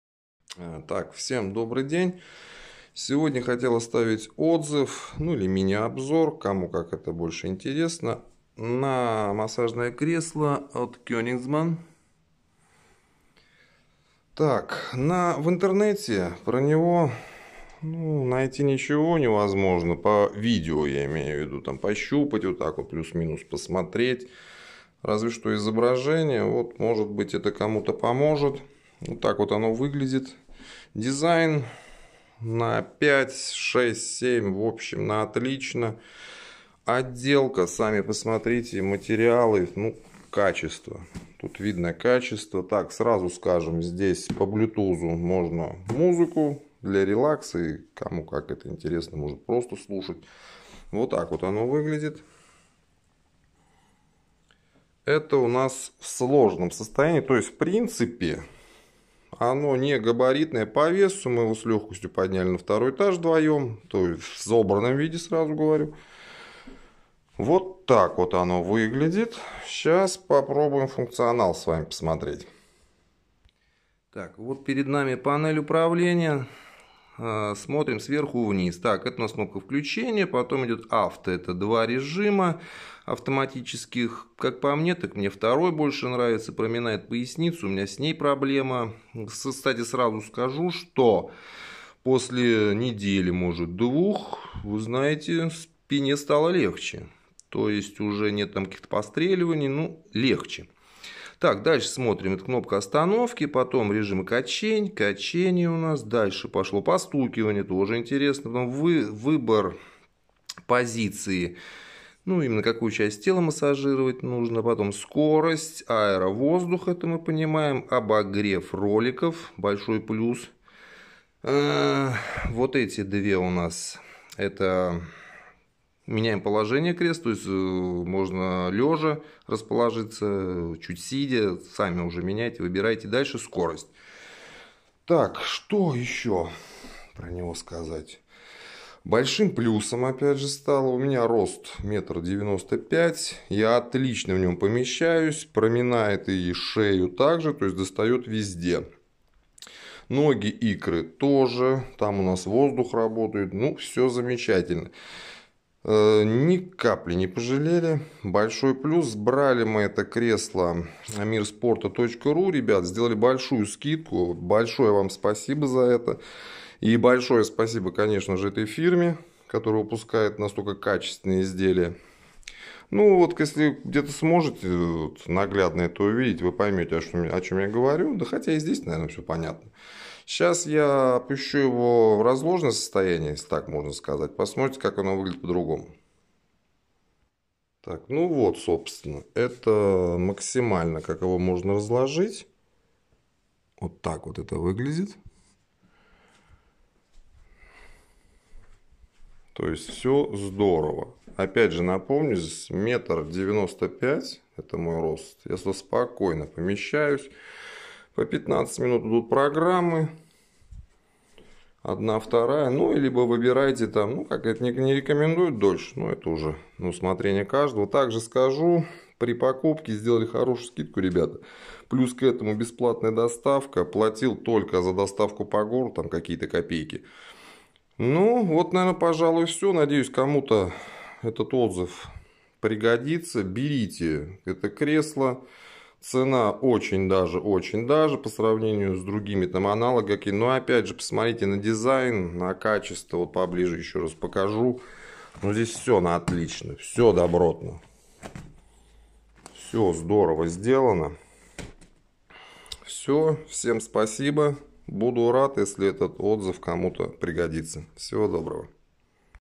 ОТЗЫВ НА МАССАЖНОЕ КРЕСЛО | KOENIGSMANN NOGO
Отзыв клиента на наше массажное кресло Koenigsmann NOGO

Изысканное массажное кресло в цветовой палитре белого жемчуга и насыщенного темного шоколада, которое подарит вам минуты незабываемого блаженства. Koenigsmann Nogo — современный дизайн, оптимальное количество массажных программ и наиболее востребованных пользователем функций. Тандем практичности, долговечности, комфорта эксплуатации.